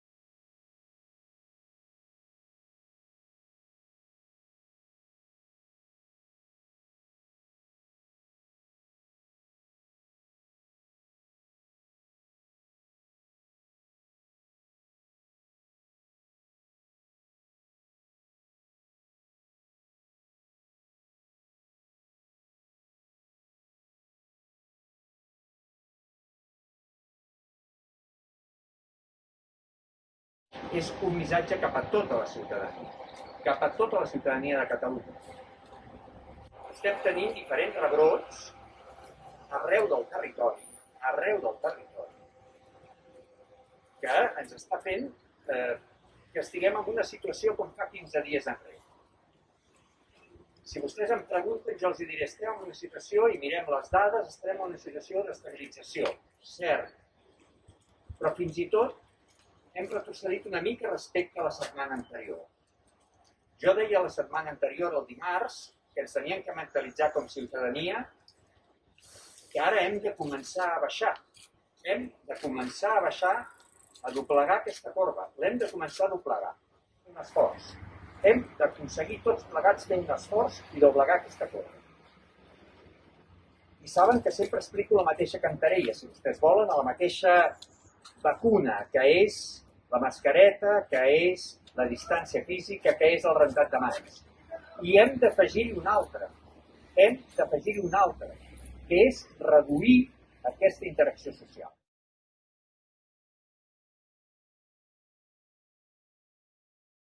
Cribratge massiu Granollers, Les Franqueses del Vallès i Canovelles
Cribratges massius a Granollers, Les Franqueses del Vallès i Canovelles per detectar nous casos de COVID-19.